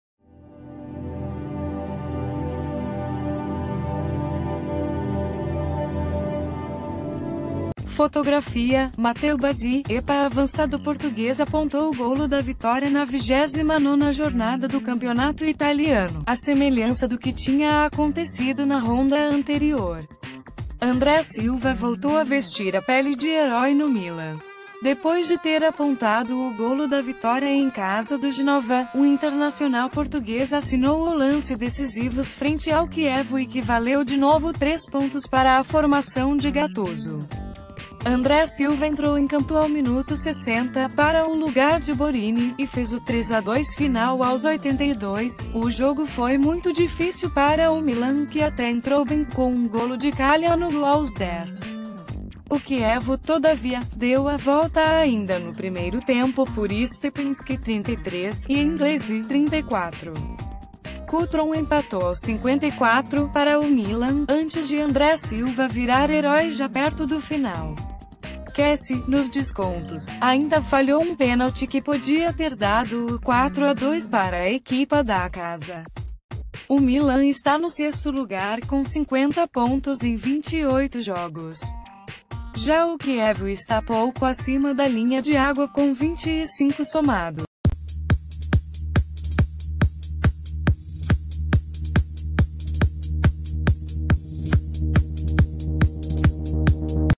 Últimas Notícias | André Silva volta a ser o herói do Milan
Fotografia: Matteo Bazzi/EPAAvançado português apontou o golo da vitória na 29ª jornada do campeonato italiano, à semelhança do que tinha acontecido na ronda anterior. André Silva voltou a vestir a pele de herói no Milan. Depois de ter apontado o golo da vitória em casa do Génova, o internacional português assinou o lance decisivo frente ao Chievo e que valeu de novo três pontos para a formação de Gattuso. André Silva entrou em campo ao minuto 60, para o lugar de Borini, e fez o 3-2 final aos 82...